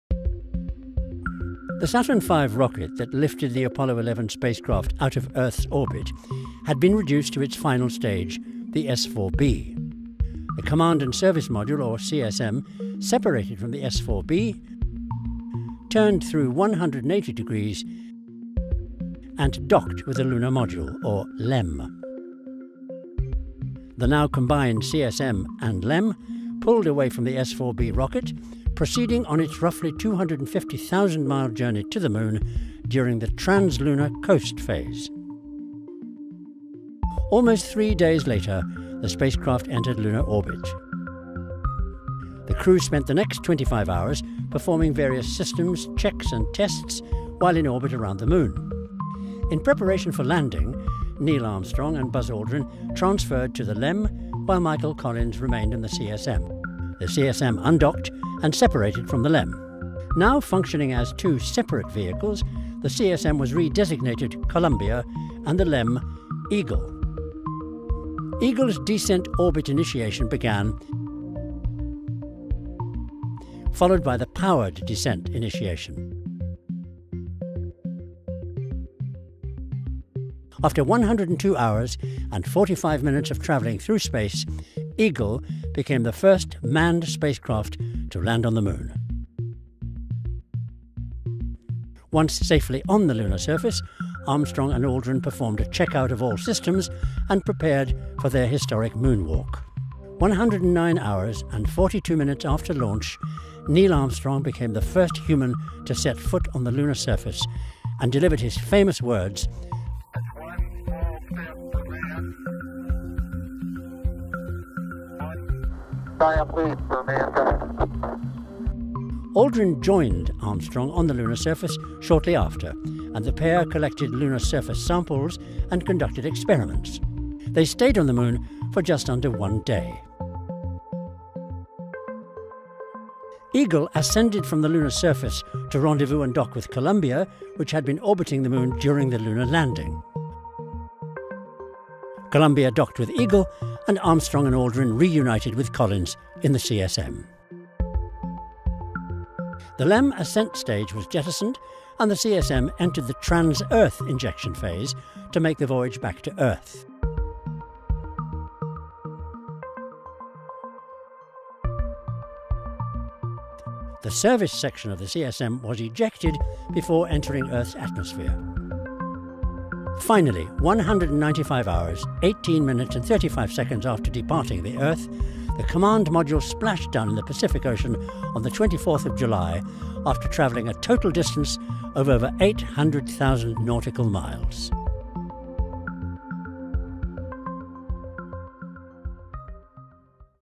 How Apollo 11 made it to the Moon and back
Getting humans to the Moon and back was a carefully choreographed journey, with the landing on the lunar surface just one part of the historic Apollo 11 mission carried out by astronauts Neil Armstrong, Buzz Aldrin and Michael Collins. Narrated by legendary BBC presenter James Burke, who led the commentary on the Moon landing, this animation shows many of the crucial steps involved for humans to walk on another world.

Credit: Science Museum